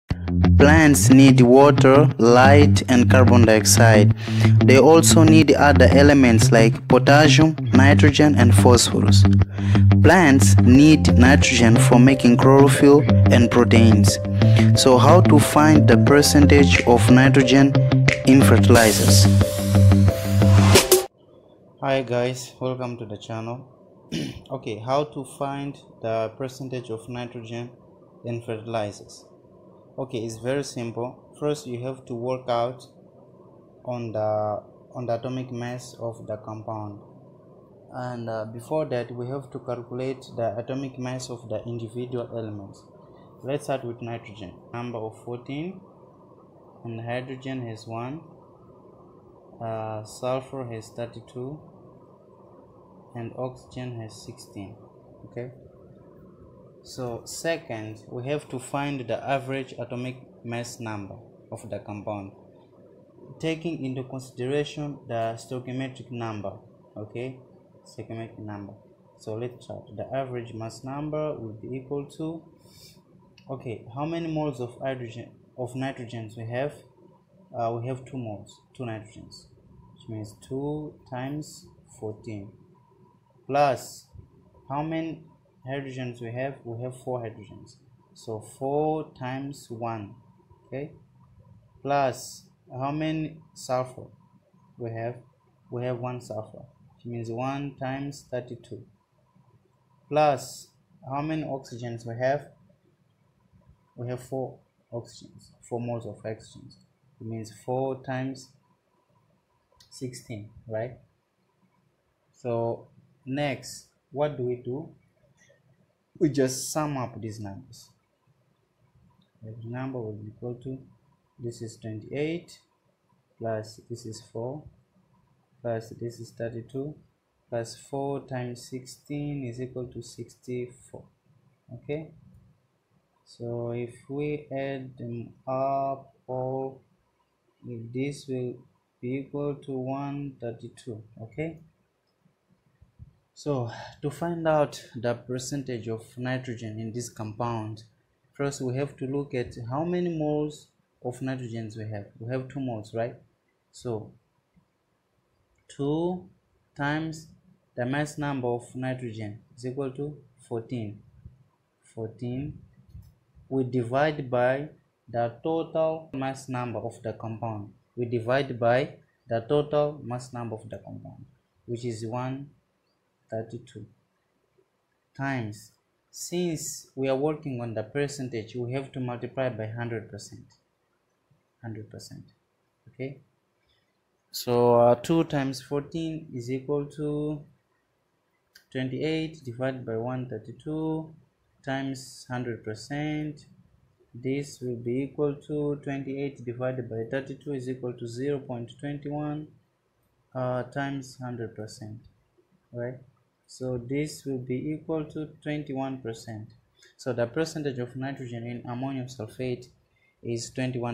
How to find the percentage of Nitrogen in Fertilizers?
A Fertilizer is any substance added to the soil to make it more fertile.
Animal manure is a natural fertilizer, synthetic fertilizers are made in factories. In How to find the percentage of Nitrogen in Fertilizers, we will know what plants need for growth, the main elements, the composition and how to find the percentage of Nitrogen in Fertilizers.